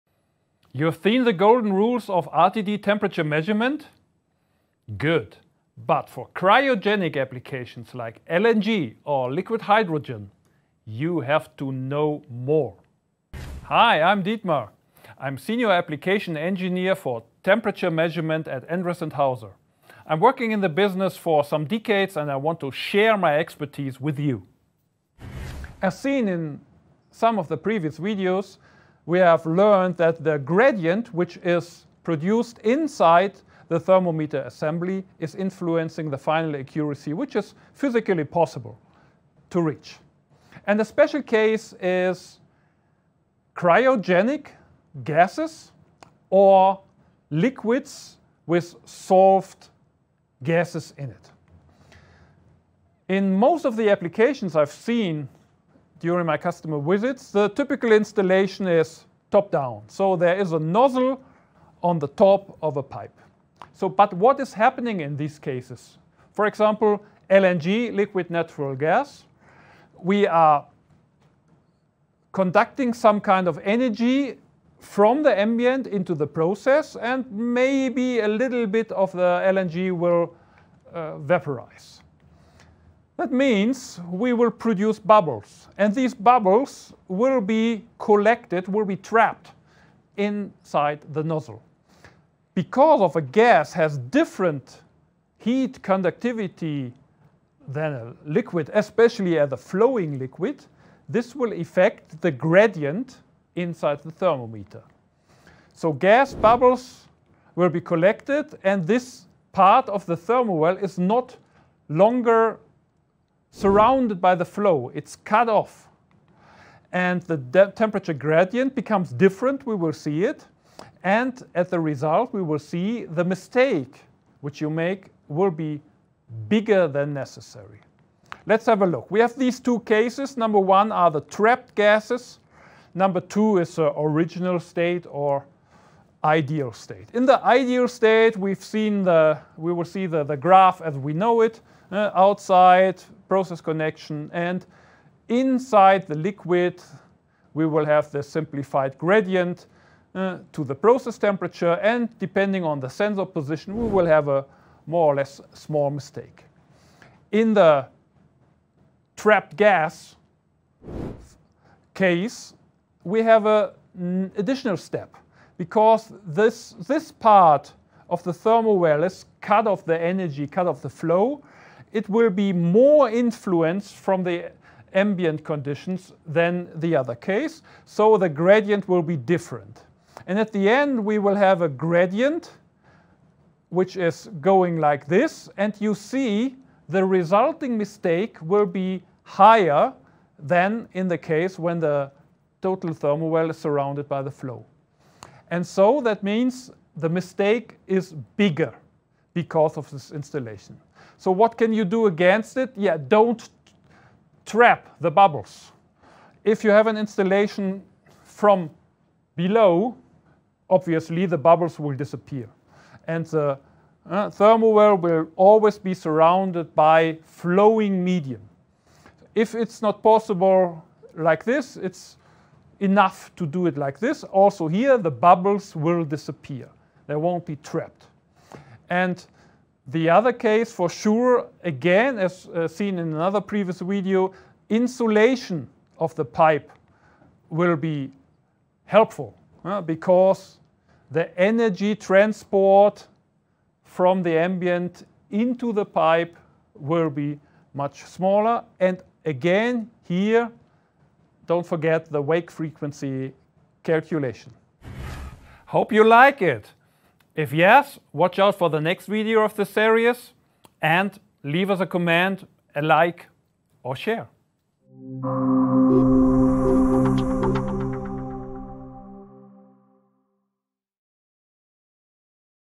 Temperature measurement in cryogenic applications
You have finished watching the golden rules of temperature measurement? Perfect! Now we look at the field of cryogenic applications and cover topics such as LNG and liquid hydrogen.

Dietmar talks about an installation in a vertical nozzle (top down, liquids and liquified gases) and explains a recommended approach to avoid mistakes in cryogenic applications:

- Under the pipe – gas and vapour bubbles will disappear
- External insulation
- Longest possible installation length – are there any elbows around?
- The resulting measurement deviation will become smaller
- Consider the results of a wake frequency calculation